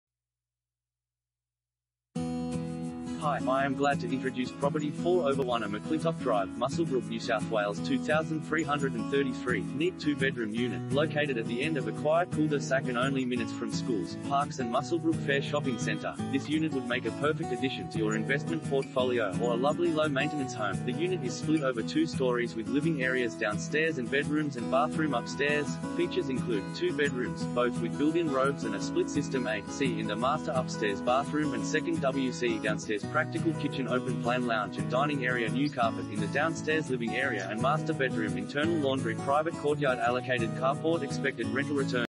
4/1A McClintock Drive, Muswellbrook NSW 2333 | AuPropertyReport.Com
4/1A McClintock Drive, Muswellbrook NSW 2333
Property Type: house
Bedrooms: 2
Bathrooms: 2
Car Space: 1
NEAT TWO BEDROOM UNIT